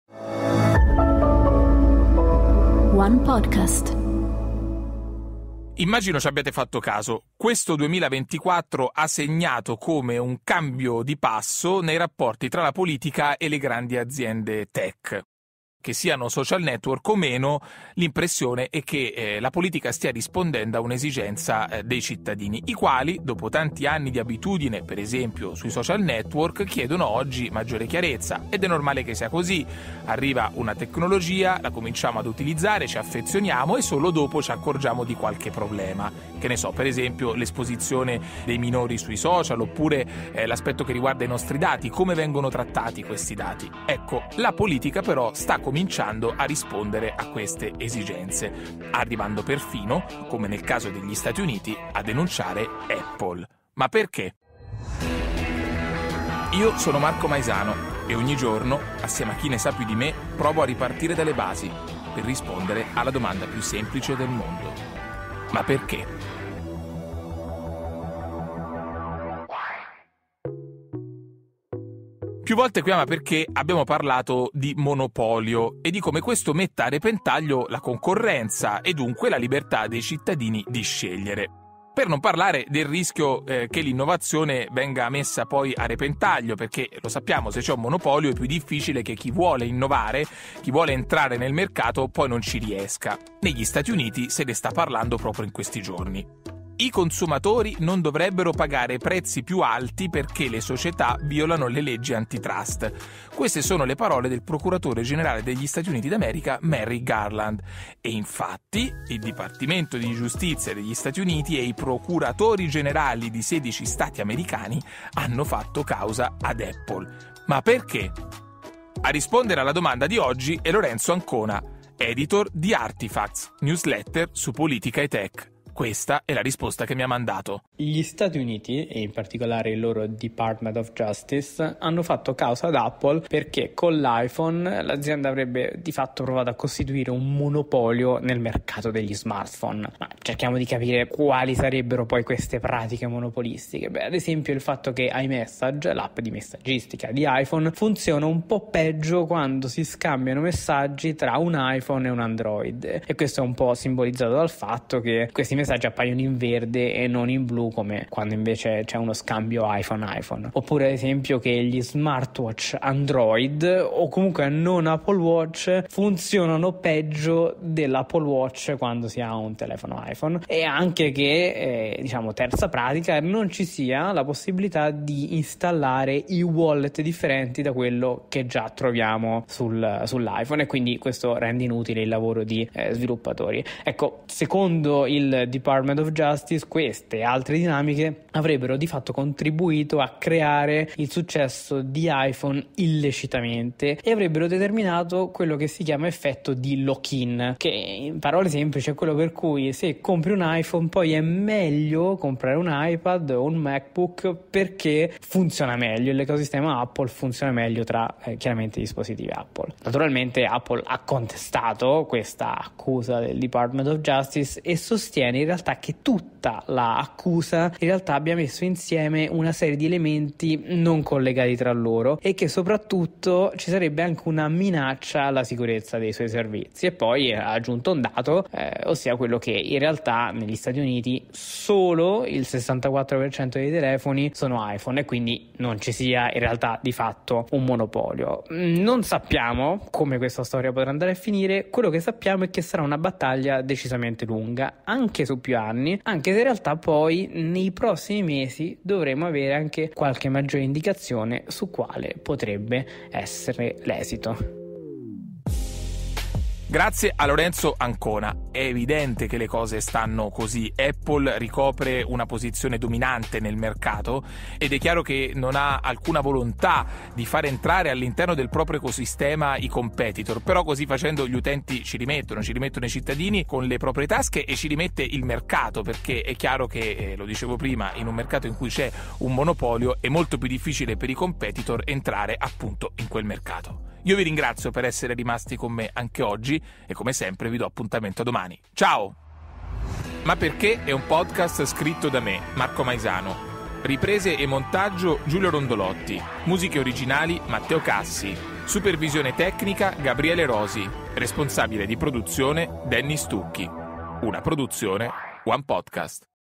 337 | Ma perché gli Stati Uniti hanno fatto causa a Apple?
Questo 2024 ha segnato come un cambio di passo nei rapporti tra politica e società tech. Ed è normale che sia così. L’innovazione arriva, e solo dopo qualche tempo i cittadini si fanno un'opinione che diventa sempre di più un sentimento comune. E appunto la politica prova a mettere in chiaro alcuni aspetti, arrivando addirittura, come nel caso degli Stati Uniti, a fare causa a Apple. Ma perché? Ne parlo con Lorenzo Ancona.